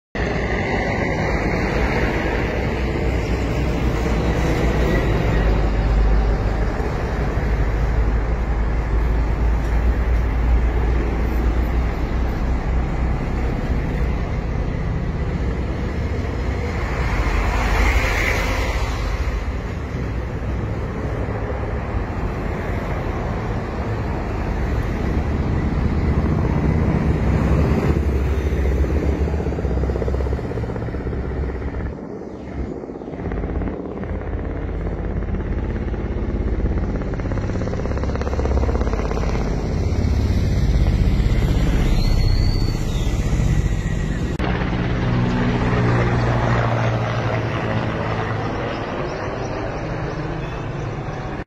“SOHR camera” captures a delegation of the UNICEF and the Syrian Red Cross heading to Allouk station
“SOHR camera” captures a joint delegation of the UNICEF and the Syrian Red Cross escorted by Russian military forces and guarded by Russian aircraft heading from Al-Darbasiyah city to Allouk water station in Ras Al-Ain countryside